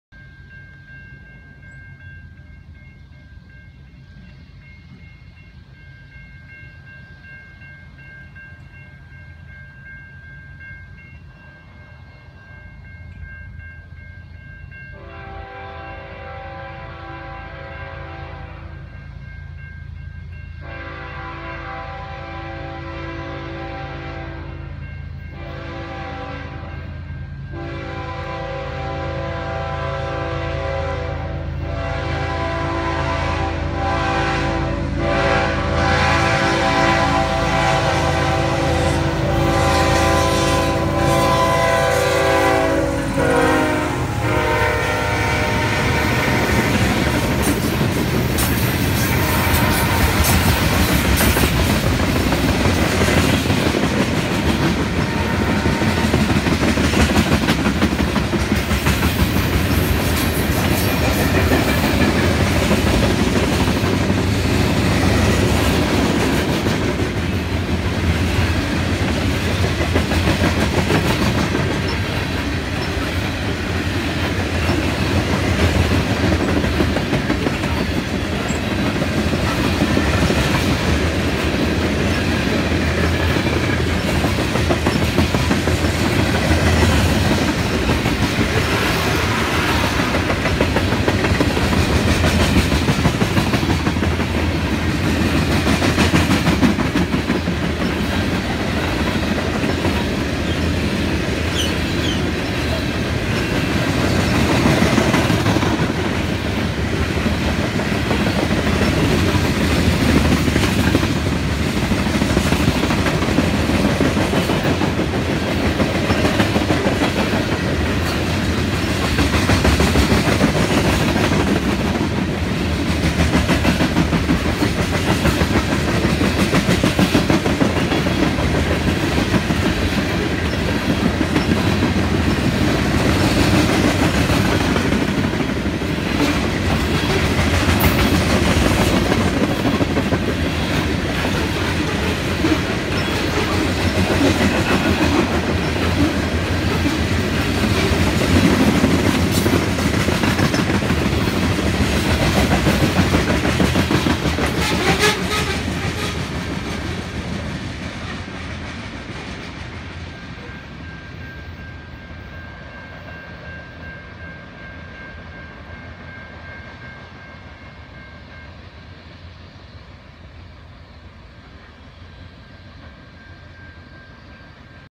Canadian National Freight Train 4
A Canadian National loaded coal train, with a Kansas City Southern secondary unit, heads southbound on Canadian National’s Champaign/Chicago Subdivision. This was filmed between the Main and Washington Street railroad crossings in Arcola, Illinois.